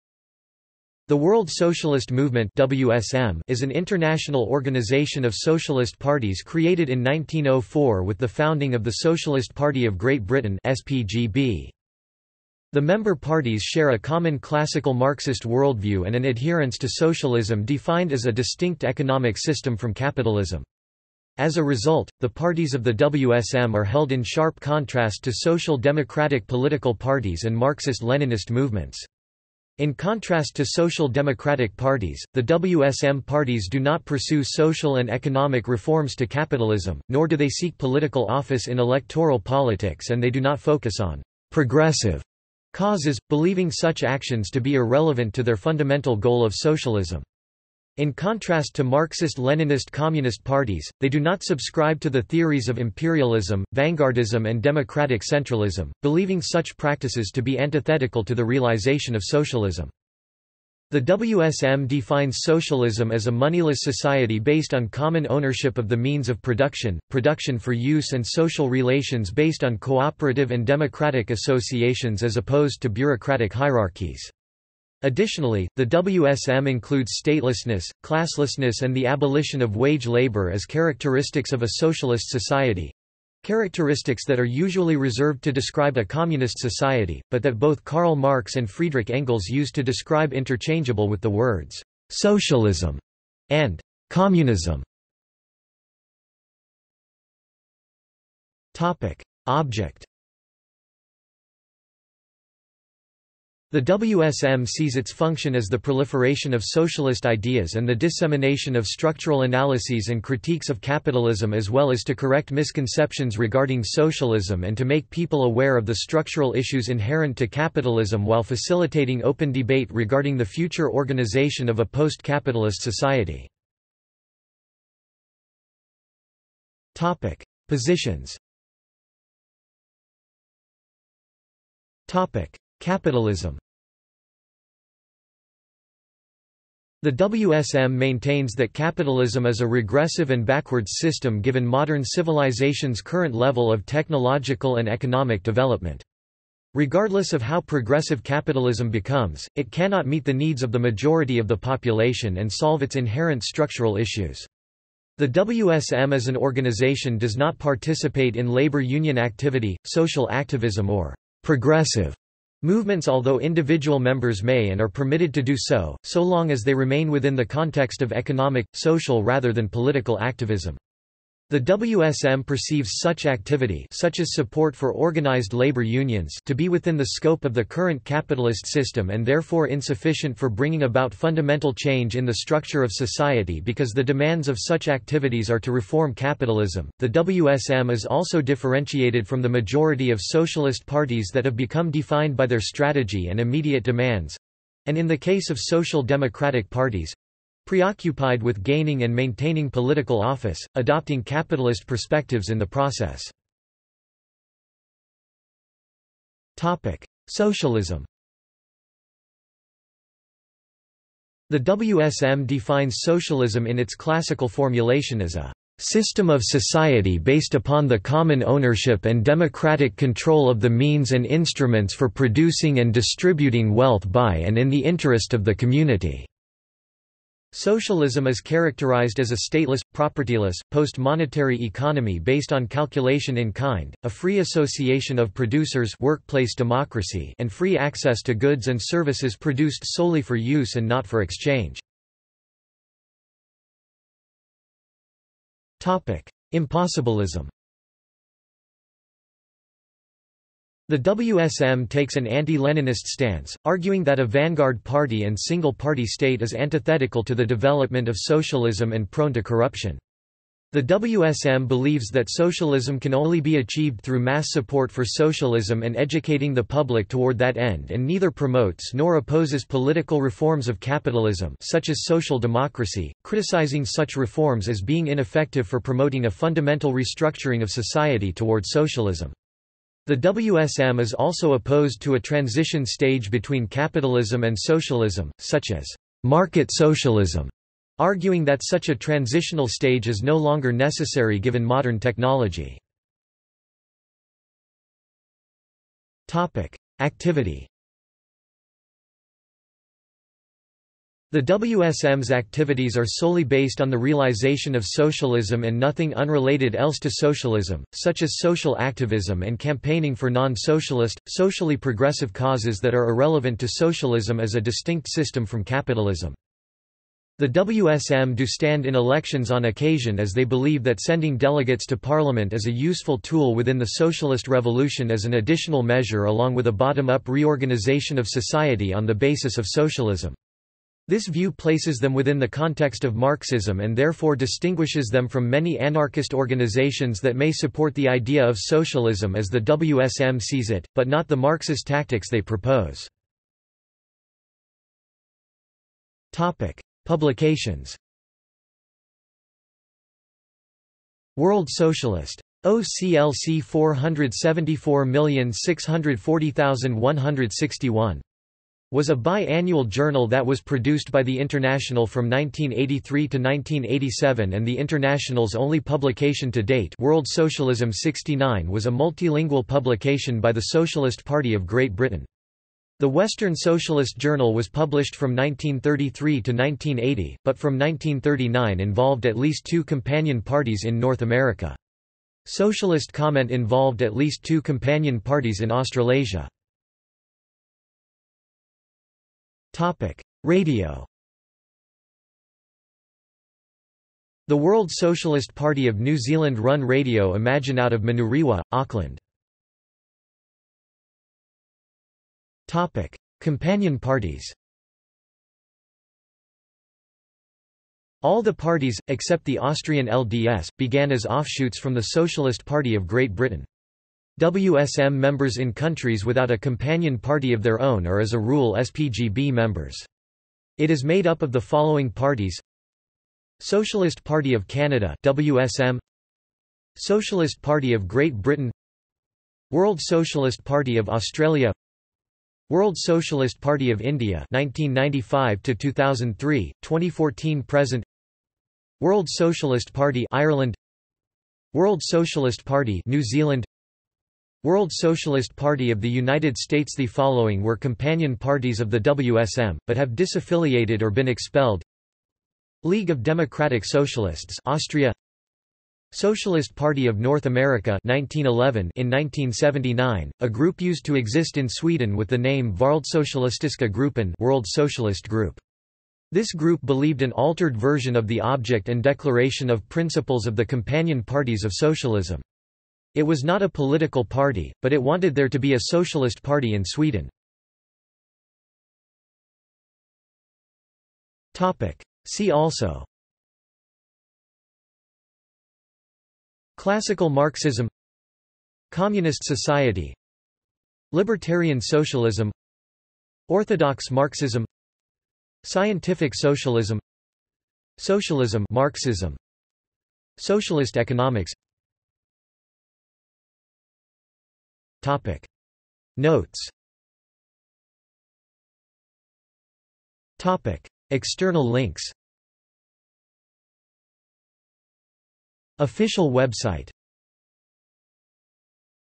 World Socialist Movement | Wikipedia audio article
This is an audio version of the Wikipedia Article:
World Socialist Movement

- Socrates

SUMMARY
The World Socialist Movement (WSM) is an international organisation of socialist parties created in 1904 with the founding of the Socialist Party of Great Britain (SPGB).
The member parties share a common classical Marxist worldview and an adherence to socialism defined as a distinct economic system from capitalism. As a result, the parties of the WSM are held in sharp contrast to social democratic political parties and Marxist–Leninist movements. In contrast to social democratic parties, the WSM parties do not pursue social and economic reforms to capitalism, nor do they seek political office in electoral politics and they do not focus on "progressive" causes, believing such actions to be irrelevant to their fundamental goal of socialism.
The WSM defines socialism as a moneyless society based on common ownership of the means of production, production for use and social relations based on cooperative and democratic associations as opposed to bureaucratic hierarchies. Additionally, the WSM includes statelessness, classlessness and the abolition of wage labor as characteristics of a socialist society—characteristics that are usually reserved to describe a communist society, but that both Karl Marx and Friedrich Engels used to describe interchangeable with the words "socialism" and "communism".